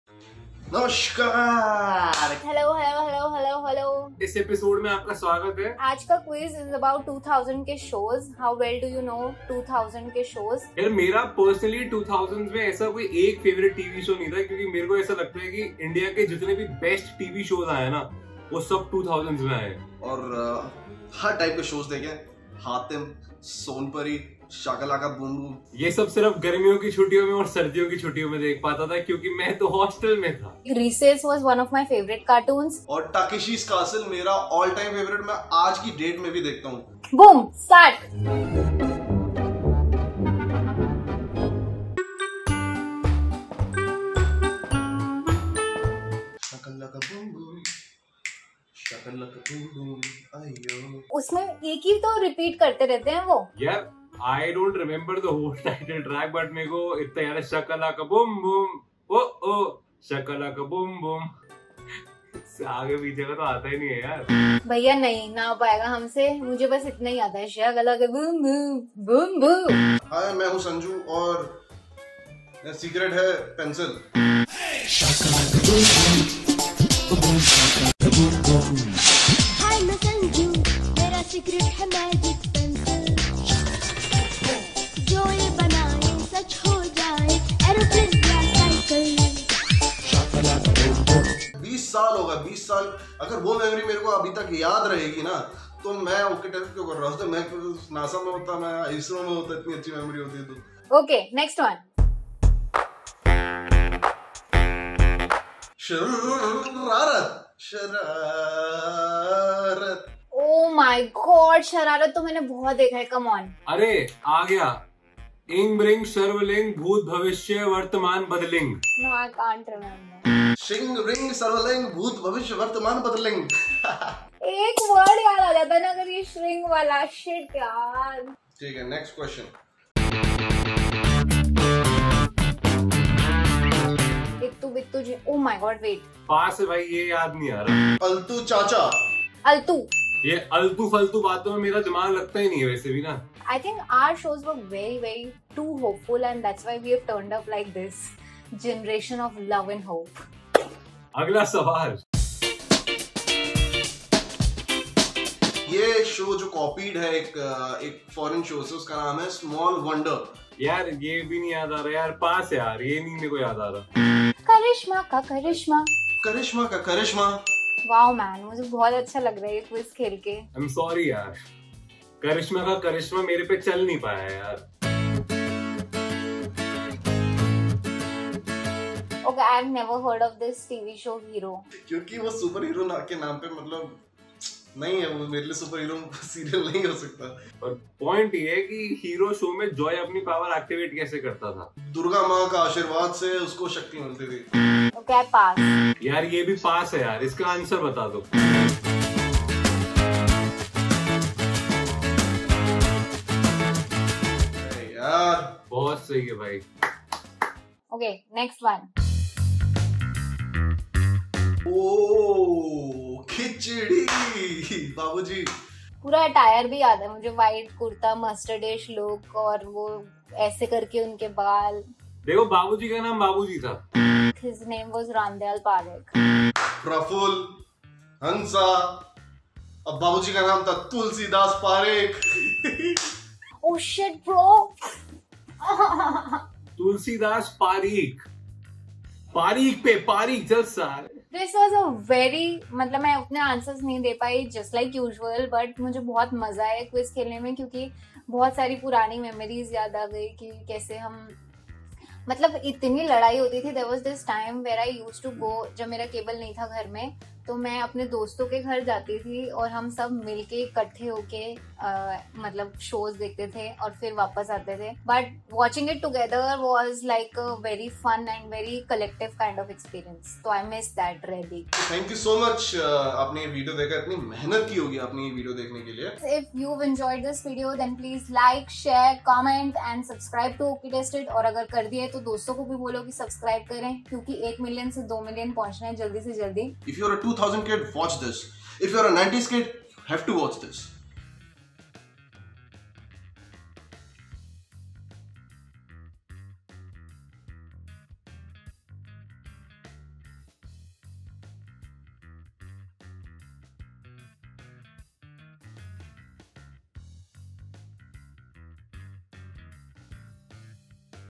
How Well Do You Know 2000s' Tv Shows? | Ok Tested
The year 2000 marked the end of a millennia but also started a long list of great shows that got us through our school and college days. The Ok Tested team tests their memories to see how well they remember the shows from that time. Find out how well you remember them as well.

For more exclusive content, download the Ok Tested app

Hit the JOIN button to support us.